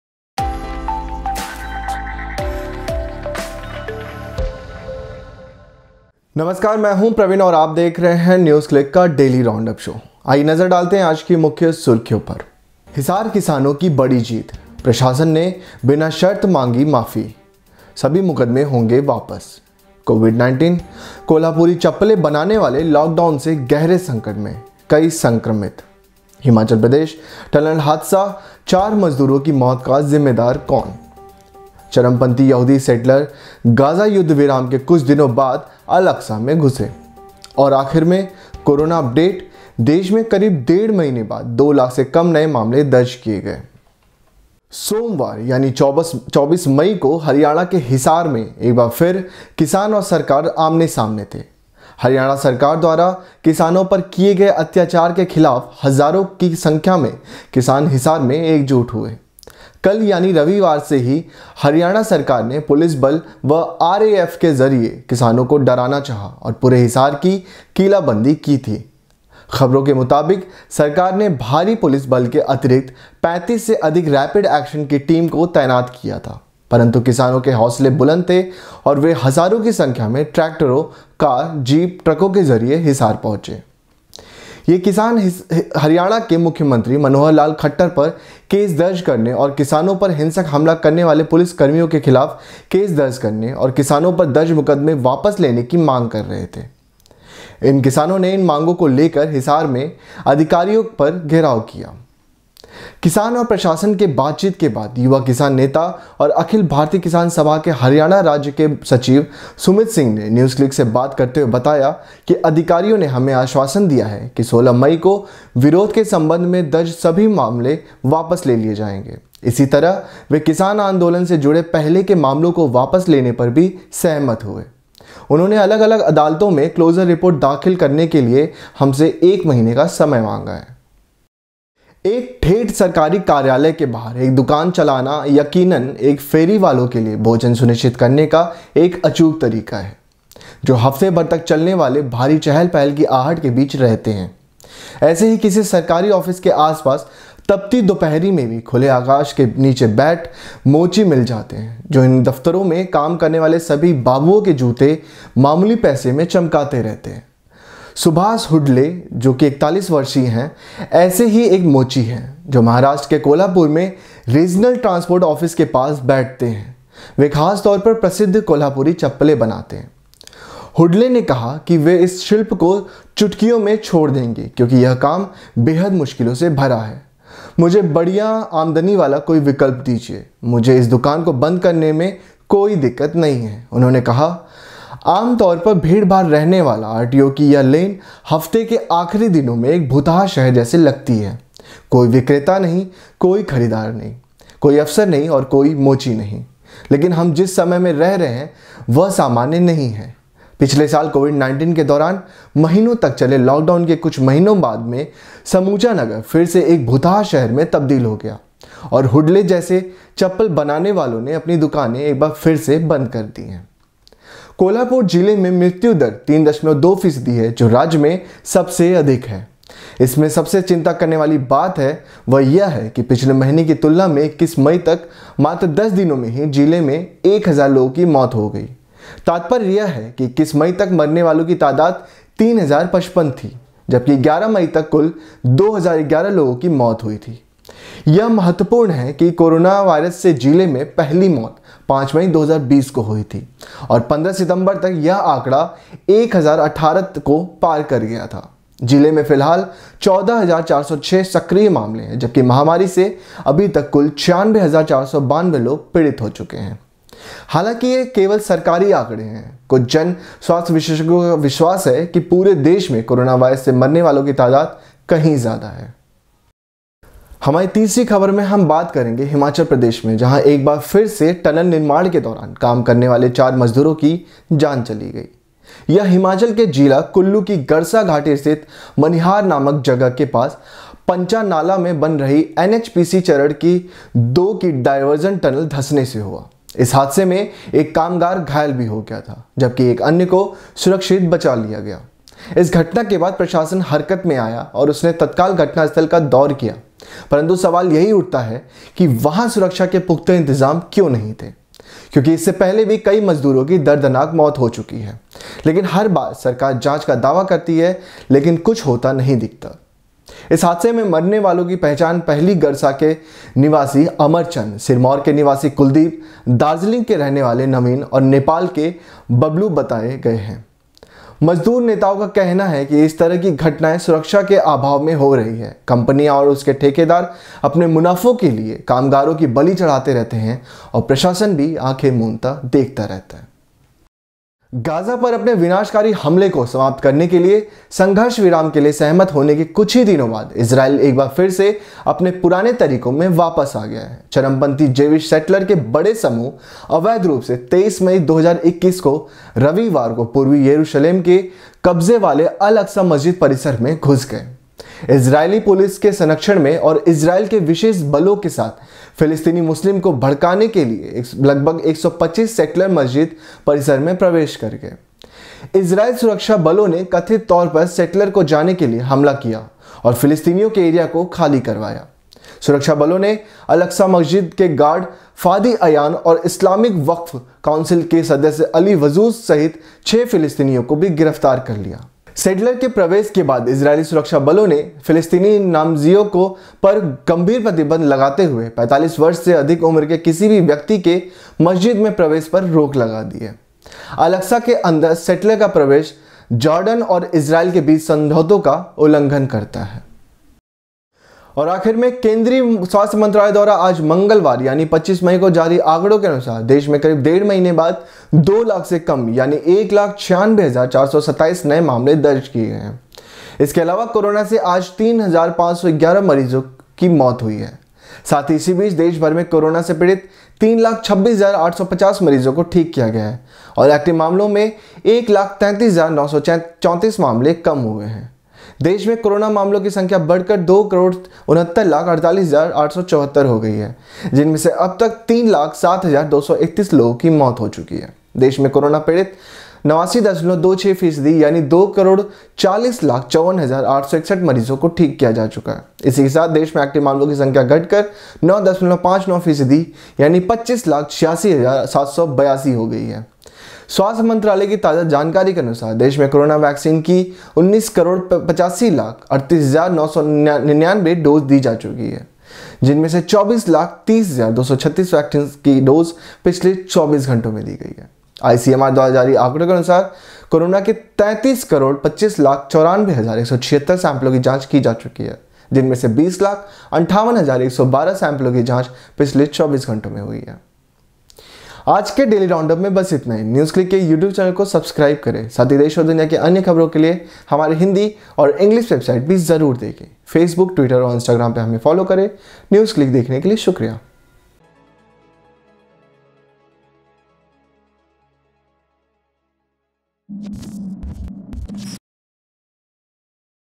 किसानों की बड़ी जीत, कोल्हापुरी चप्पलें बनाने वाले लॉकडाउन से गहरे संकट में और अन्य
आज के डेली राउंडअप में हम बात करेंगे हिसार किसान आंदोलन की।  साथ ही हमारी नज़र होगी देश और विदेश की अन्य महत्वपूर्ण ख़बरों पर